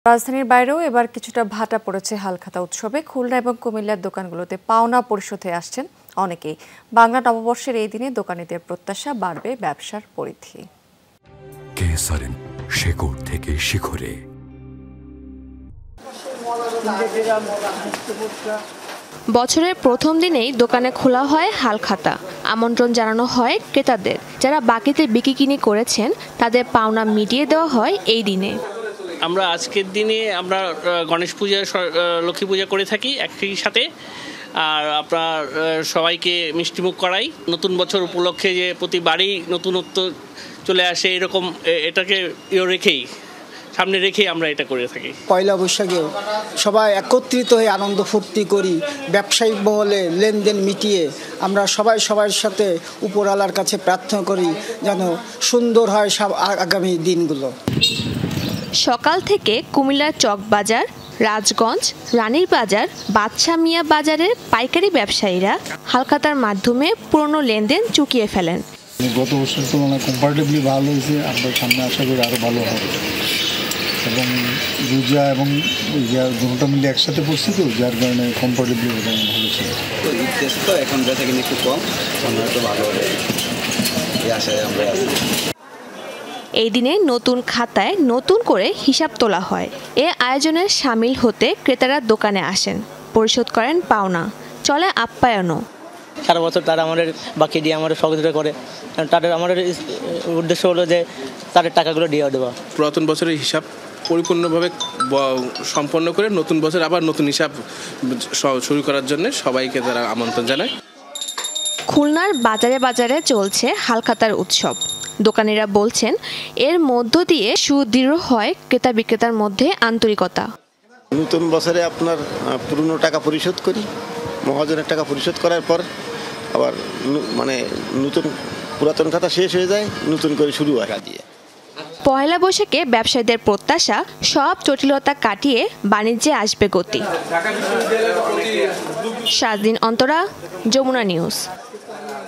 রাজধানীর বাইরে কী অবস্থা হালখাতা উৎসবের? | Dhaka Outside Halkhata | Jamuna TV
রাজধানীর বাইরেও এবার কিছুটা ভাটা পড়েছে হালখাতা উৎসবে। খুলনা ও কুমিল্লার দোকানগুলোতে পাওনা পরিশোধে আসছেন অনেকে। বাংলা নববর্ষের এই দিনে দোকানীদের প্রত্যাশা, বাড়বে ব্যবসার পরিধি।

Address:
Bangladesh.